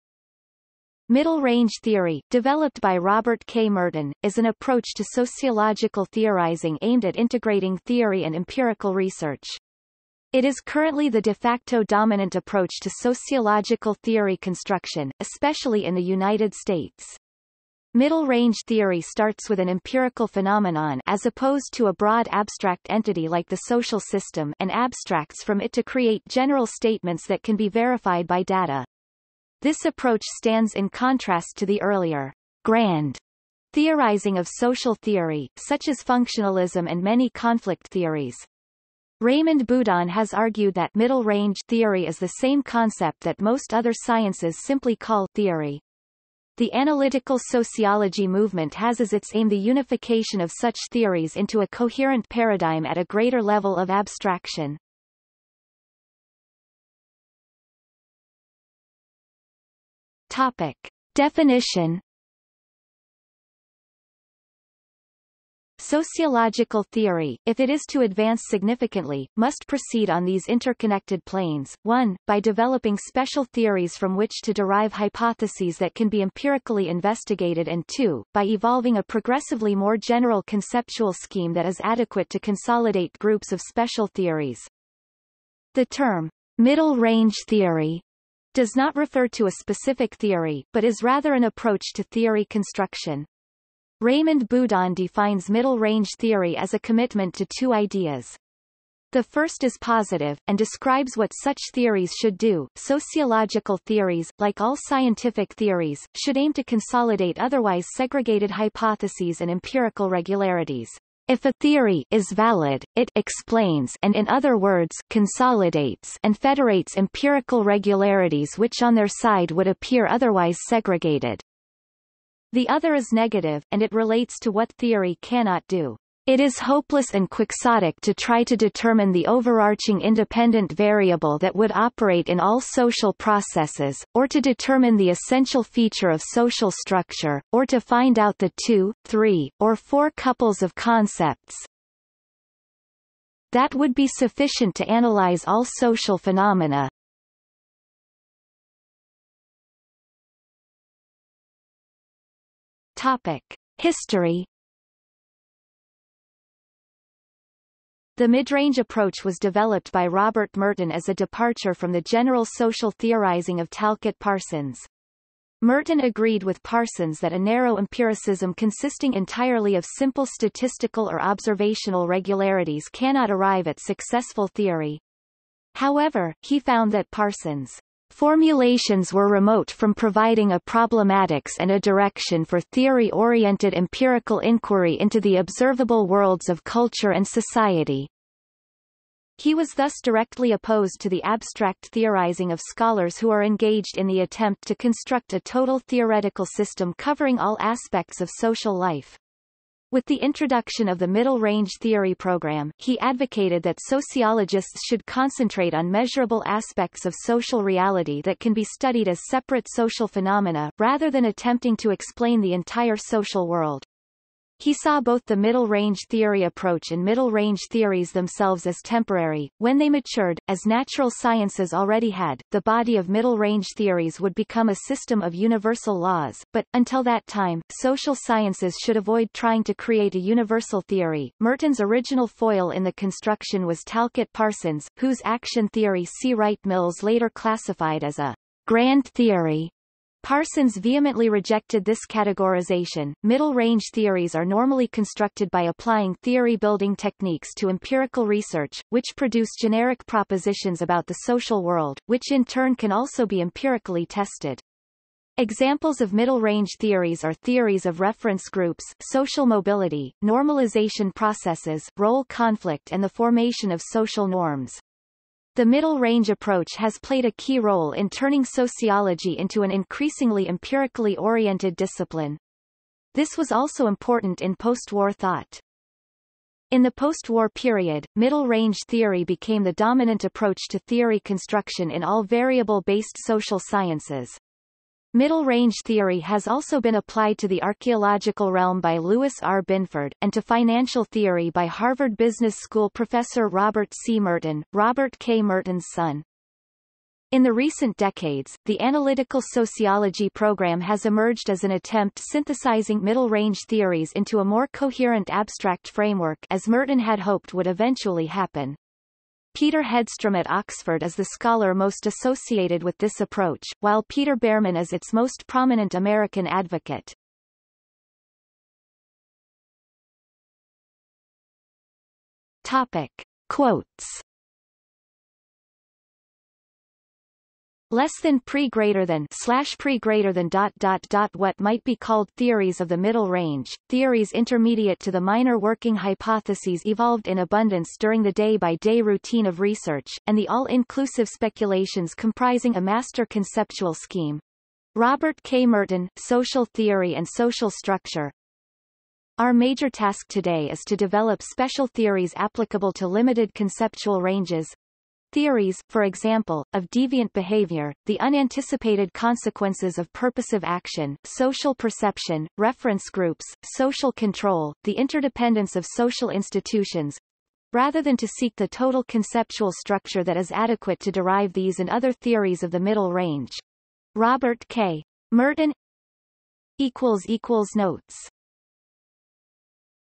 Middle range theory (sociology) | Wikipedia audio article
00:00:56 1 Definition
00:02:29 2 History
00:05:19 3 Quotes

- Socrates

SUMMARY
Middle-range theory, developed by Robert K. Merton, is an approach to sociological theorizing aimed at integrating theory and empirical research. It is currently the de facto dominant approach to sociological theory construction, especially in the United States. Middle-range theory starts with an empirical phenomenon (as opposed to a broad abstract entity like the social system) and abstracts from it to create general statements that can be verified by data. This approach stands in contrast to the earlier "grand" theorizing of social theory, such as functionalism and many conflict theories. Raymond Boudon has argued that 'middle-range' theory is the same concept that most other sciences simply call 'theory'. The analytical sociology movement has as its aim the unification of such theories into a coherent paradigm at a greater level of abstraction.